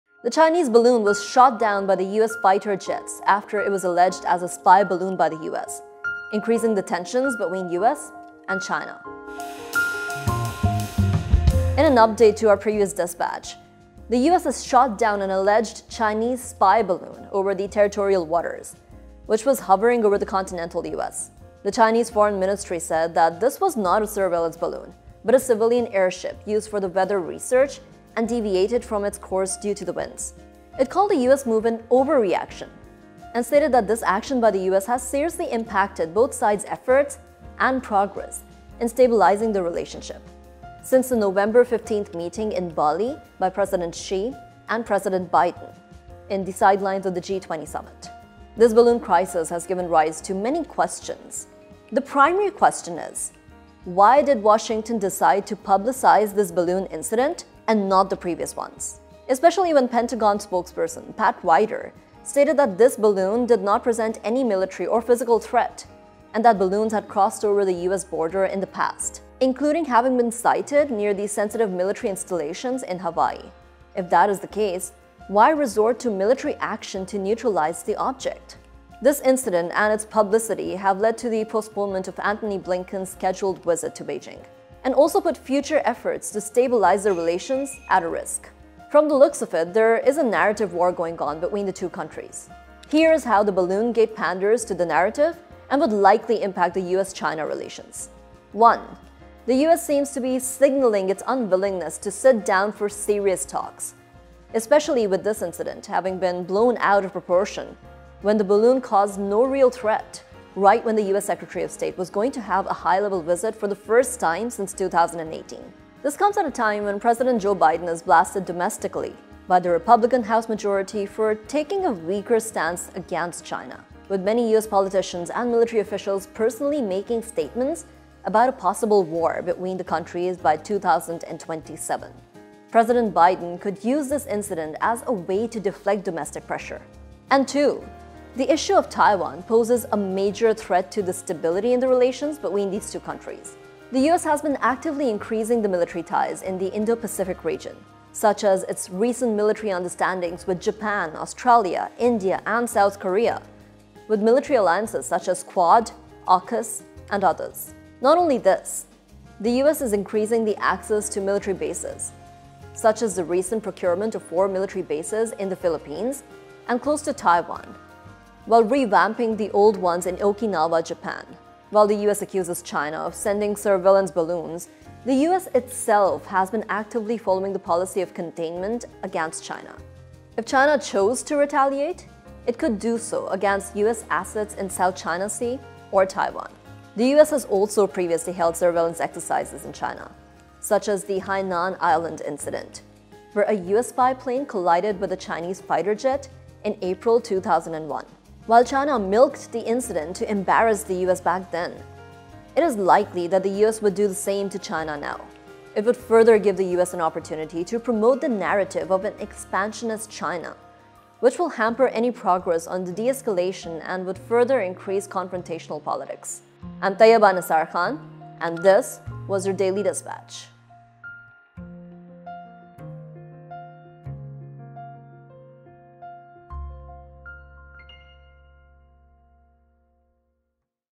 Chinese balloon shot down by US fighter jet after allegation of surveillance
In an update to our previous dispatch, the US has shot down an alleged Chinese spy balloon over territorial waters, which was hovering over the continental US. The Chinese foreign ministry said that this was not a surveillance balloon, but a civilian airship used for weather research and deviated from its course due to the winds. It called the US move an ‘overreaction’ and stated that this action by the US has seriously impacted both sides’ efforts and progress in stabilizing the relationship since the November.

Tune in as we break down news stories from near and far, and infer the world around us.

On-Screen: Tayyaba Nisar Khan
Research: Tayyaba Nisar Khan
Production: Arooj Abrar